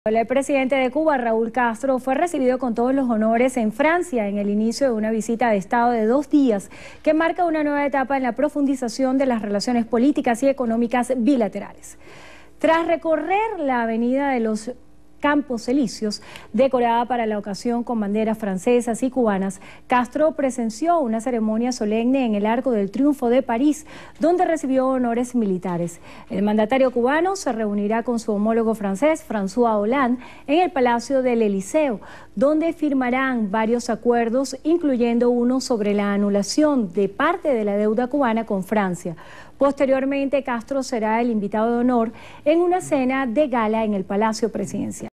Francia recibe con todos los honores al presidente de Cuba Raúl Castro
Suscríbete El presidente de Cuba, Raúl Castro, fue recibido este lunes con todos los honores en Francia en el inicio de una visita de Estado de dos días que marca una nueva etapa en la profundización de las relaciones políticas y económicas bilaterales. El dirigente cubano llegó a París el fin de semana; pero, su agenda oficial comenzó este lunes